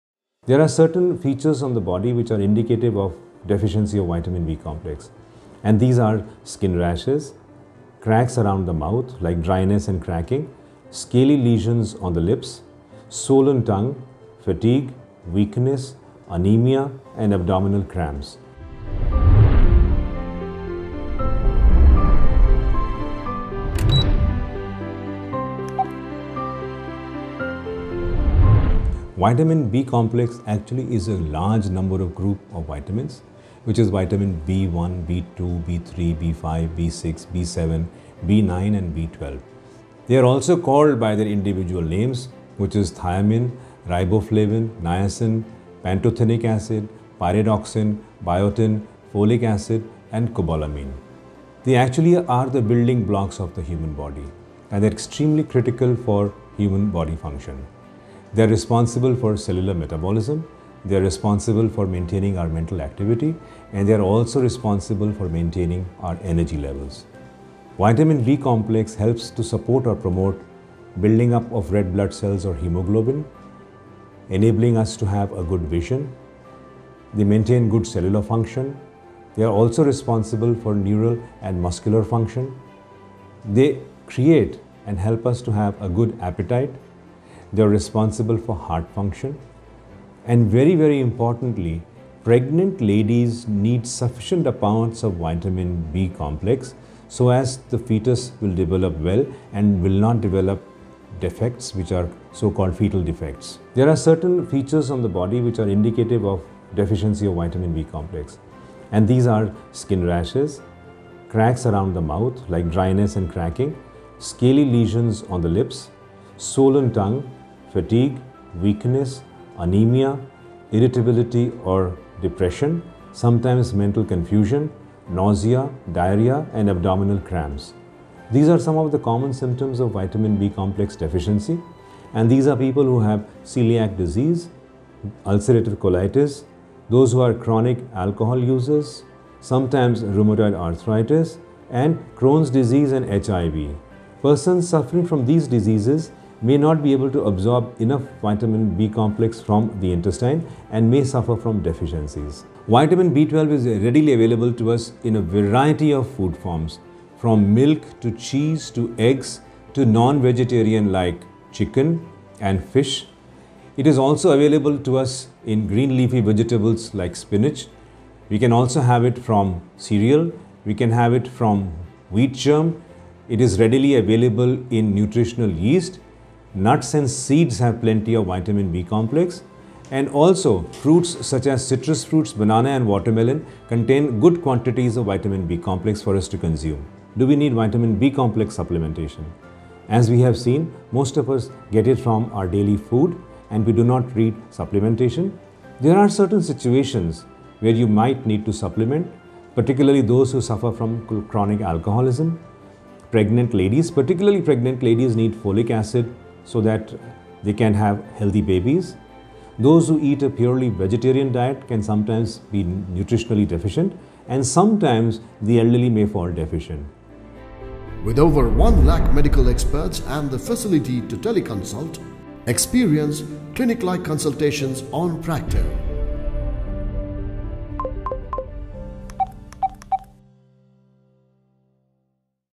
Vitamin B Deficiency: Symptoms, Causes, Sources, Treatment || Vitamin B Rich Foods || Practo
What are the benefits of Vitamin B? What are the symptoms of Vitamin B deficiency? What are Vitamin B rich foods? Are there any easy sources of Vitamin B complex? What are the types of Vitamin B? Who is prone to Vitamin B deficiency? Dr Rajesh Bhardwaj, a senior ENT specialist, tells us all about Vitamin B and its supplementation.

Video Breakdown:

0:33 What is Vitamin B

0:55 Importance of Vitamin B

1:45 Vitamin B Deficiency

2:16 Prone to Vitamin B Deficiency

2:34 Sources of Vitamin B

3:10 Vitamin B Supplementation

Video Transcript:

Introduction
Sometimes, certain people may be deficient in Vitamin B. There are some signs that are
indicative of a deficiency of Vitamin B complex. A simple blood test is done to determine your
serum B complex levels and that will tell you whether you are deficient and if you&#39;re deficient
you can take supplementation!

Understanding Vitamin B
Vitamin B Complex is a large group of vitamins - Vitamin B1, B2, B3, B5, B6, B7, B9 and B12.
They are also called by their individual names Thiamin, Riboflavin, Niacin, Pantothenic Acid,
Pyridoxine, Biotin, Folic Acid and Cobalamin.

Importance of Vitamin B
They are all important for bodily functions. They are the building blocks of the human body and
are critical for the human body function. They are responsible for cellular metabolism,
maintaining mental activities and energy levels. Vitamin B complex helps build up red blood
cells or hemoglobin, enables us to have good vision, maintains good cellular function, and are
responsible for neural and muscular functions. They create and help us to have a good appetite.
They are responsible for heart function. Very importantly, pregnant women need a sufficient
amount of Vitamin B Complex for the fetus to develop well, without defects.

Signs of deficiency
These are skin rashes, cracks around the mouth like dryness and cracking scaly lesions on the
lips, swollen tongue, fatigue, weakness, anaemia, irritability or depression, sometimes mental
confusion, Nausea, diarrhea and abdominal cramps. These are some of the common
symptoms of vitamin B complex deficiency.

People who are prone to Vitamin B deficiency
People who are more at risk of developing vitamin B complex deficiency are those who have
celiac disease, ulcerative colitis, chronic alcohol users and sometimes, people with rheumatoid
arthritis and Crohn&#39;s disease and HIV. People with these diseases may not be able to absorb
enough vitamin B complex from the intestine and they suffer from deficiencies.

Sources of Vitamin B
Vitamin B12 is readily available to us in a variety of food forms from milk, cheese, eggs and
meat. It is also available to us in green leafy vegetables like spinach. You can also get it from
cereals like wheat germ. It is readily available in nutritional yeast, nuts and seeds, which have
plenty of vitamin B complex. Also, fruits such as citrus fruits, banana and watermelon contain
good quantities of Vitamin B complex.

Do we need vitamin B complex supplementation?
As we have seen most of us get it from our daily food and we do not need supplementation.
There are certain situations where you might need to supplement particularly those who suffer
from chronic alcoholism. Additionally, pregnant ladies may need extra folic acid so that they can
have healthy babies. Those who eat a purely vegetarian diet can sometimes be nutritionally
deficient and sometimes they may be deficient in Vitamin B.